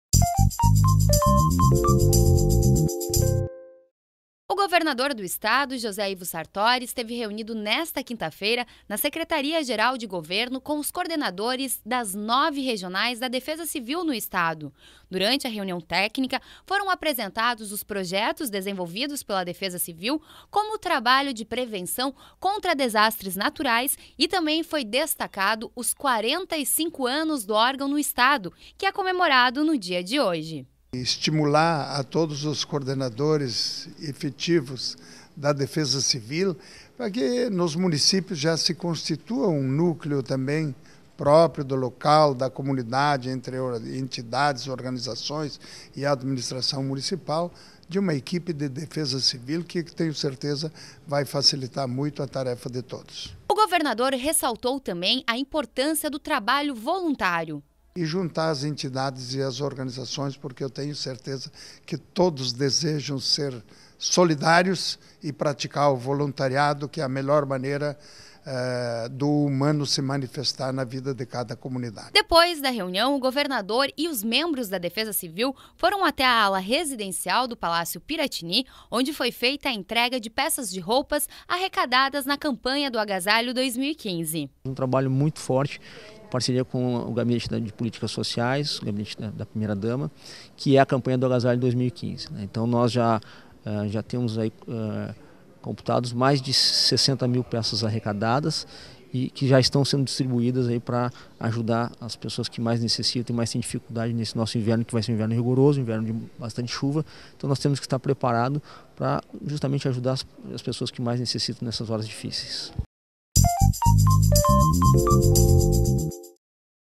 Dia Estadual da Defesa Civil é lembrado com encontro técnico e doação de agasalhos
Nesta quinta-feira (9), é comemorado o Dia Estadual da Defesa Civil no Rio Grande do Sul.
No encontro, com a presença do governador José Ivo Sartori, também foram apresentadas as ações e os projetos realizados pela Defesa Civil neste ano, bem como o que está previsto para o segundo semestre.